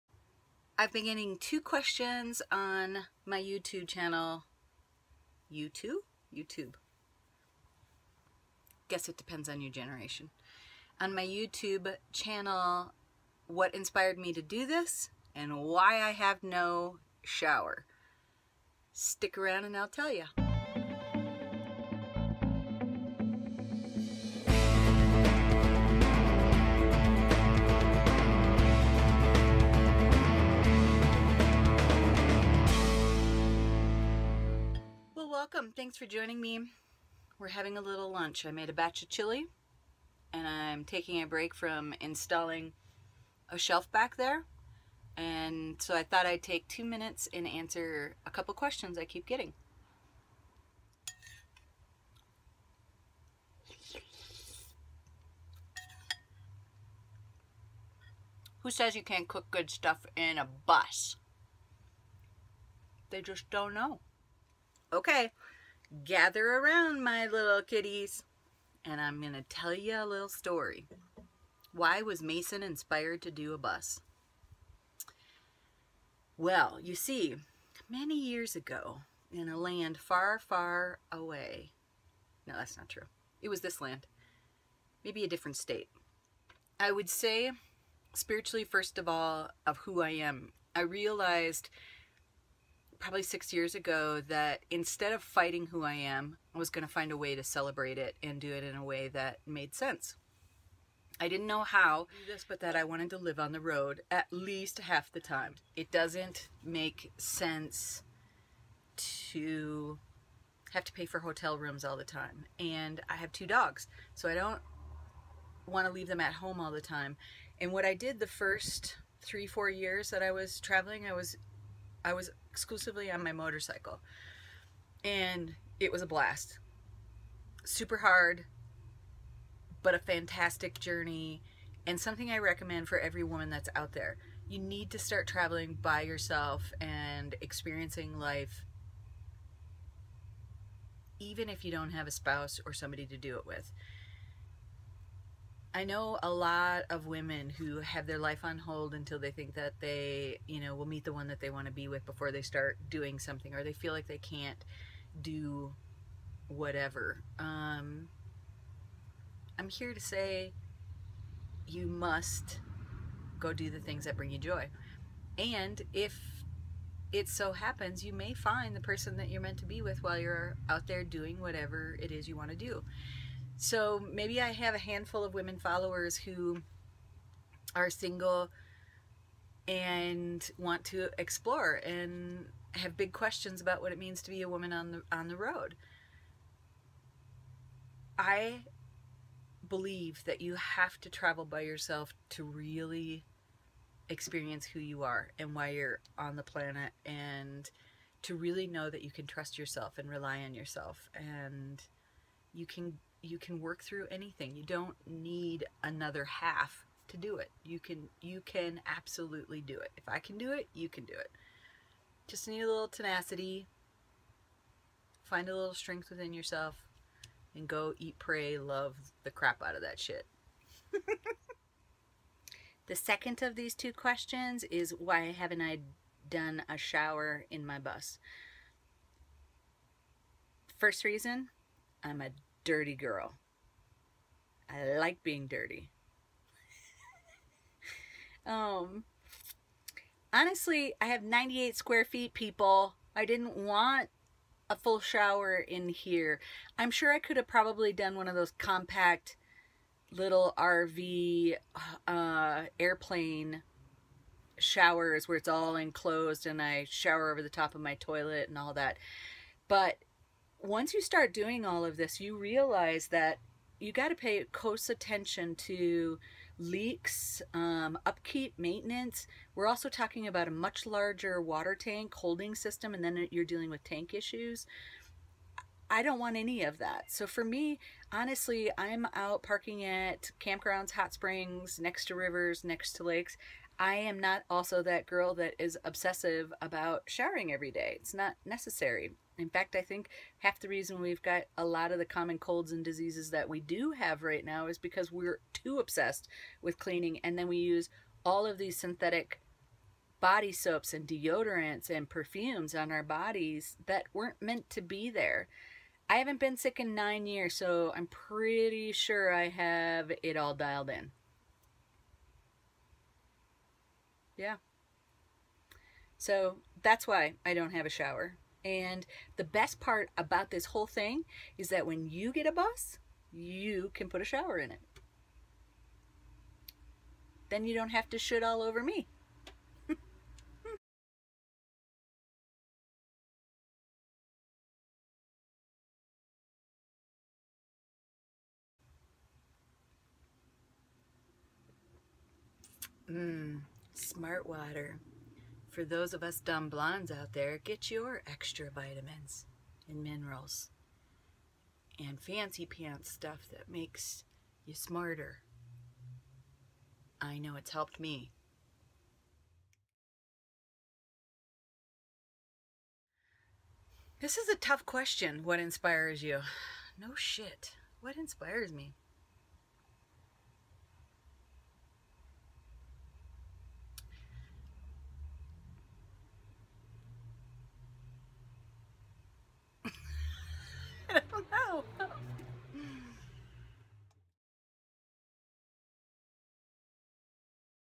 Short Bus Q&A Round 1: Without A Plot
I'll be making quick down and dirty Q&A video's answering viewers questions. Here are the answers to the first questions I got asked the most last week!  Just having a little fun with this whole thing and I hope you do too!!